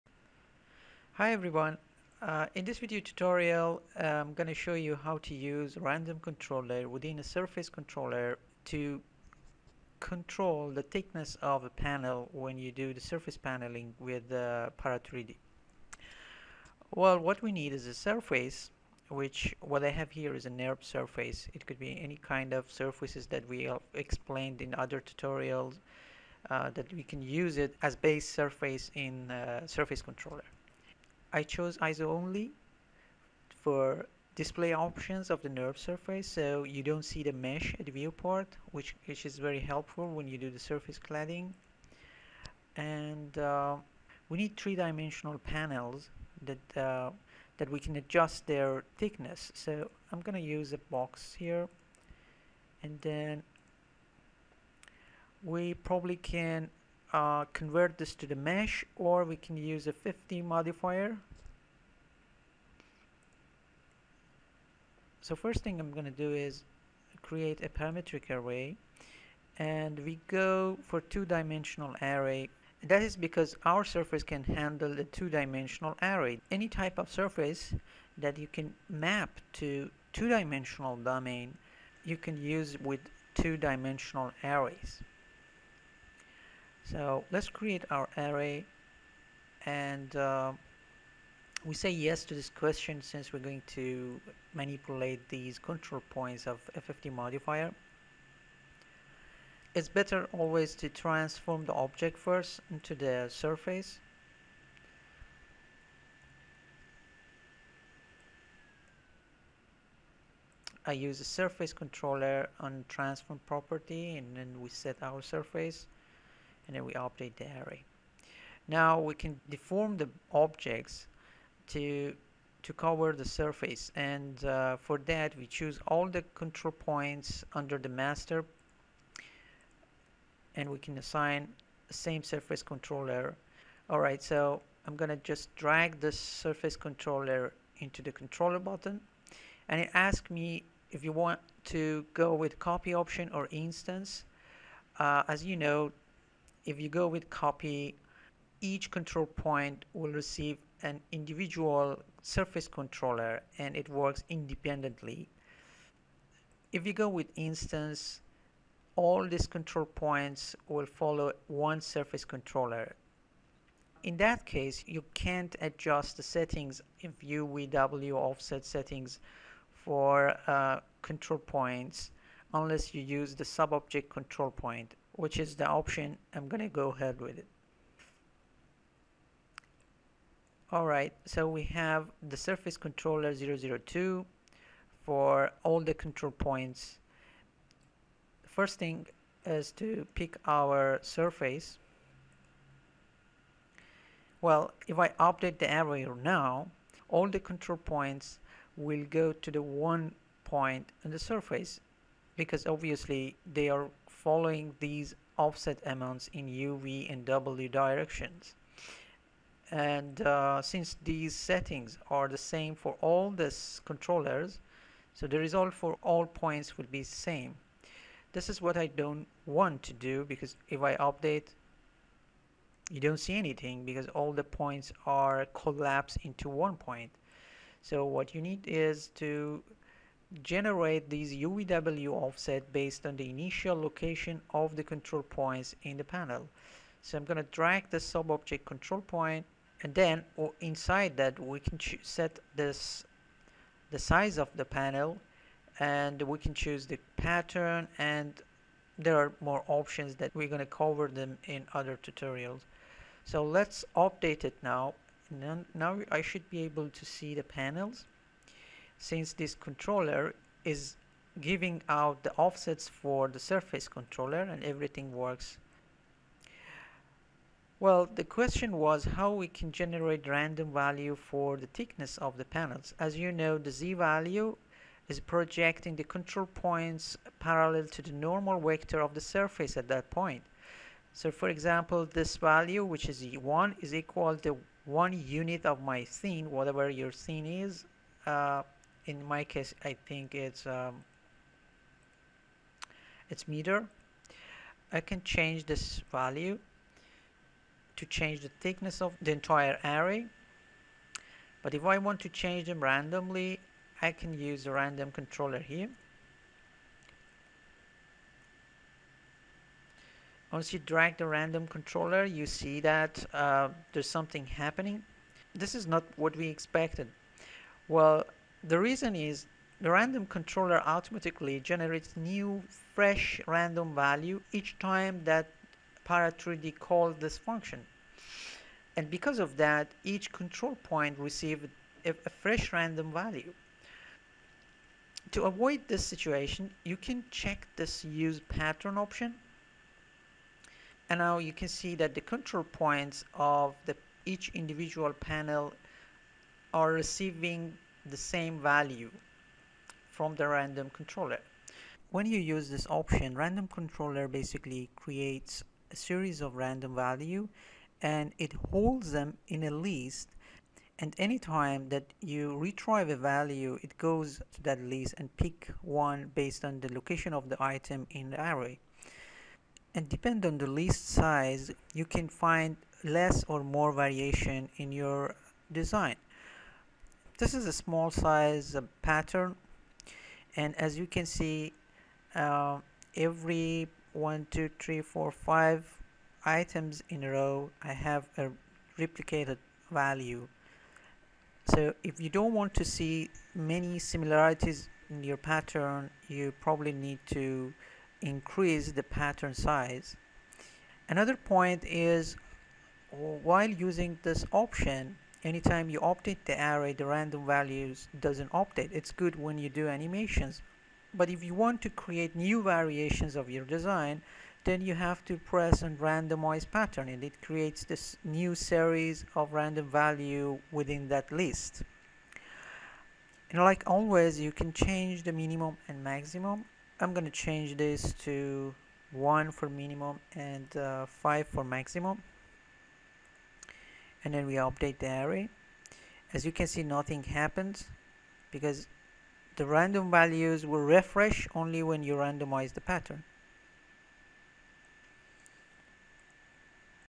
Randomizing thickness in surface paneling
In this video you learn how to use random controller within surface controller to change the thickness of panels. Also you learn how to control the random values while using random controller on multiple properties/controllers